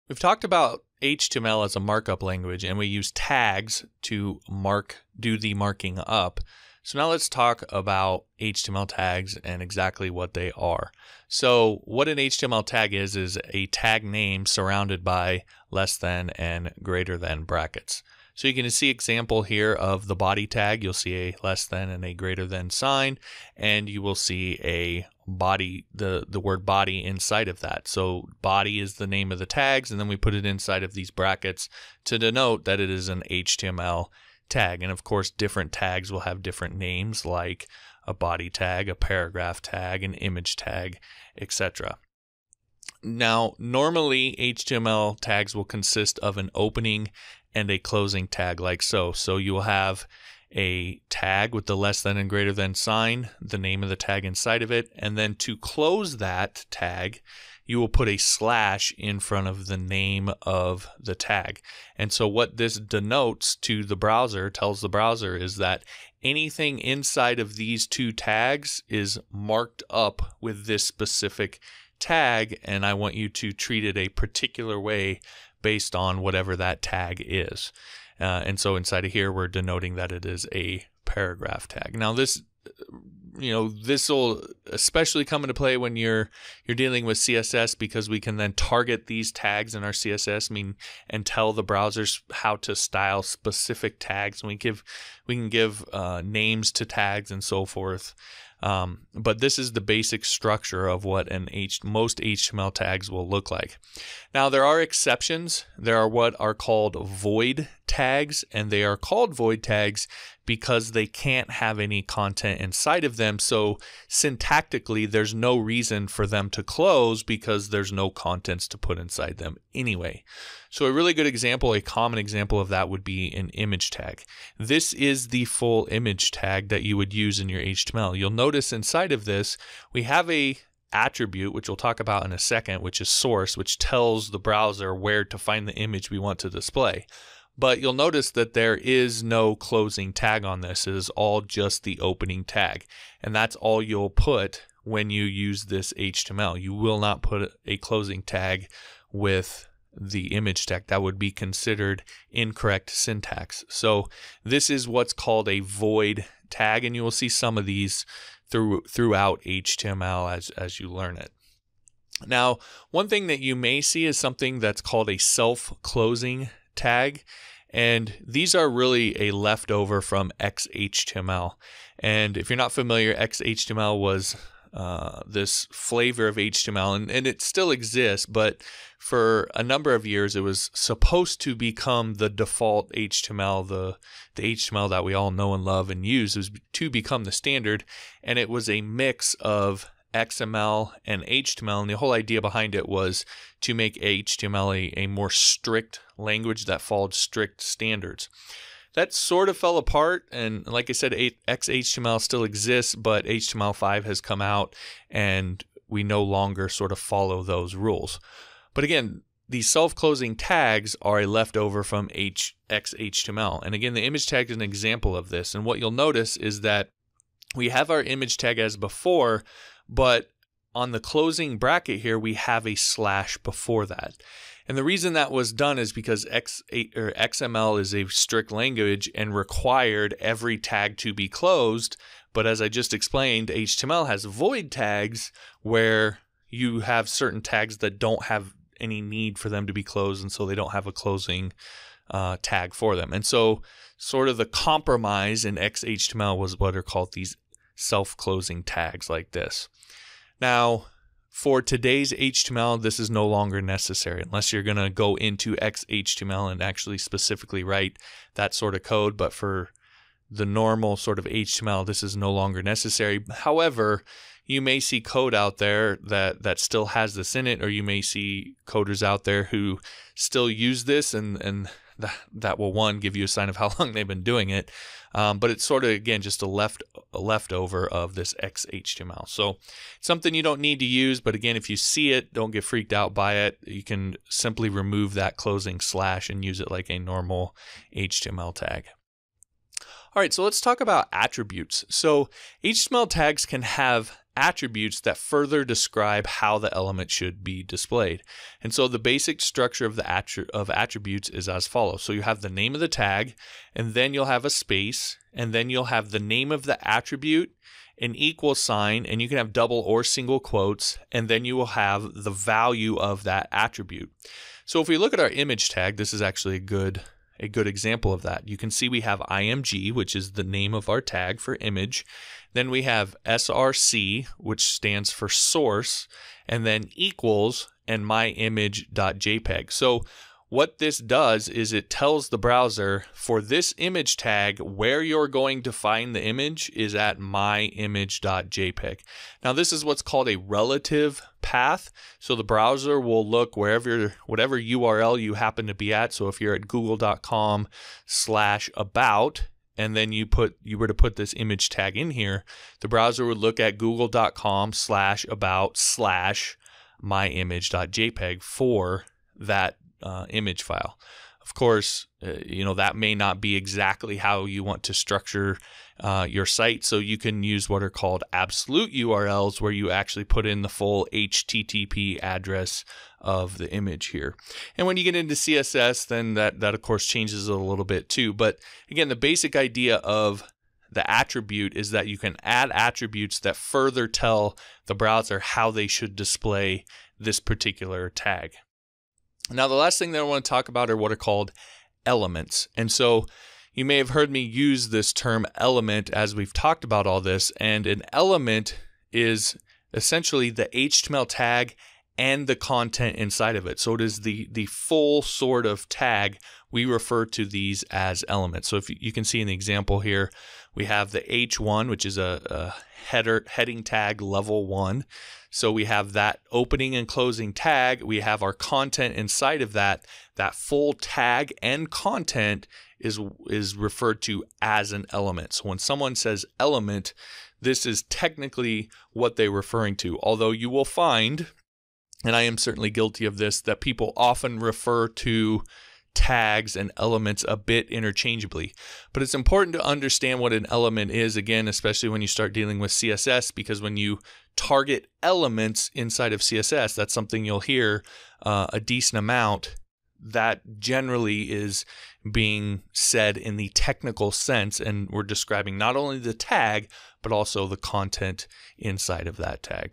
HTML Tags, Attributes and Elements
An HTML tutorial for beginners. In this lesson, learn the difference between HTML tags, attributes and elements.